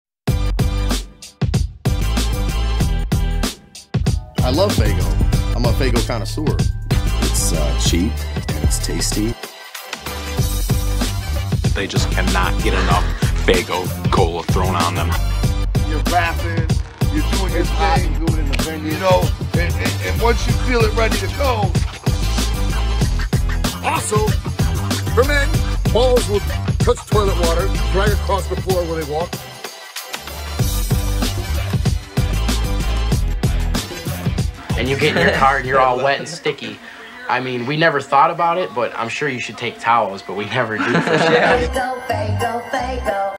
DRINK FAYGO (sponsored content)
I was paid exactly 48.63 for this ad. Faygo, I thank you for the opportunity.